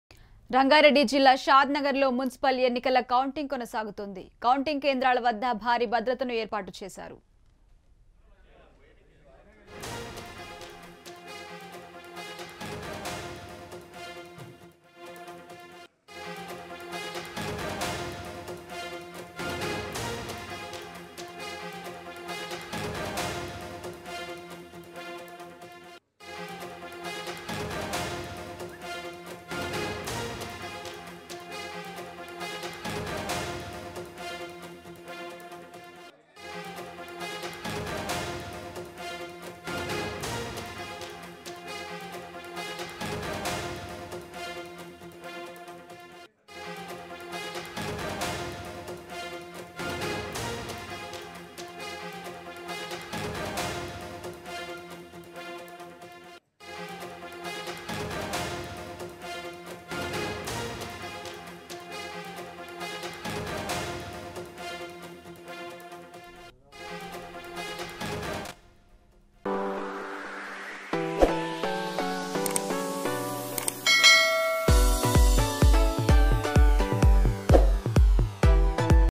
రంగారెడ్డి జిల్లా షాద్ నగర్ లో మున్సిపల్ ఎన్నికల కౌంటింగ్ కొనసాగుతంది..! | Sneha TV
రంగారెడ్డి జిల్లా షాద్ నగర్ లో మున్సిపల్ ఎన్నికల కౌంటింగ్ కొనసాగుతంది..!